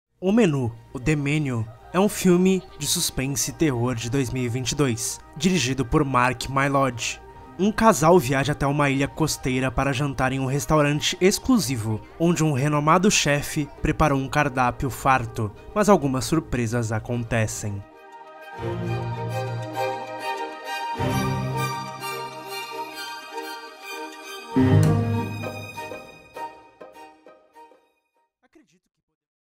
O MENU | VÍDEO COMPLETO NO CANAL
Faço uma pequena análise do filme O Menu (The Menu), dirigido por Mark Mylod.